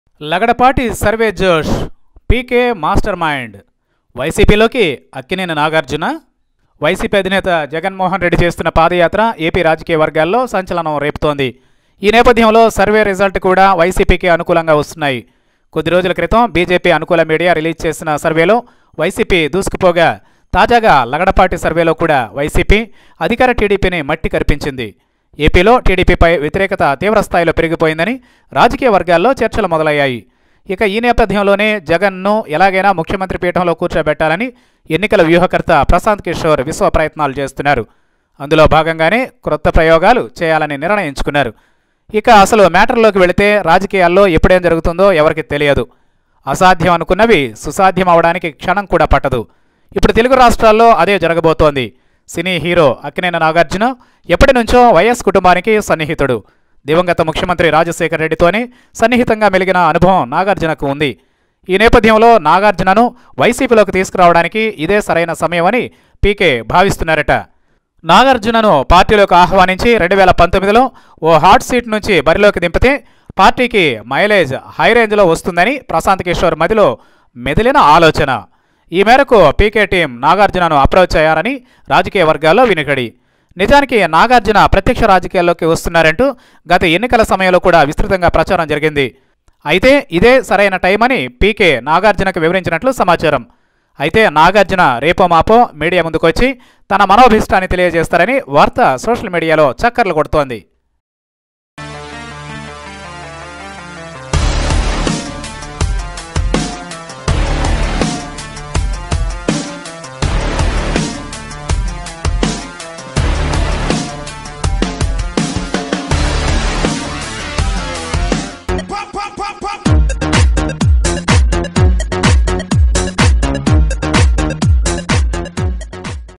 ప్రశాంత్ కిషోర్ ప్లాన్..వైసీపీలో చేరనున్న నాగార్జున| Hero Nagarjuna to Join YCP soon..!
Hero Nagarjuna to Join YCP soon..!
Tollywood Hero Nagarjuna to join YCP with the Effort of Party Adviser Prashant Kishor.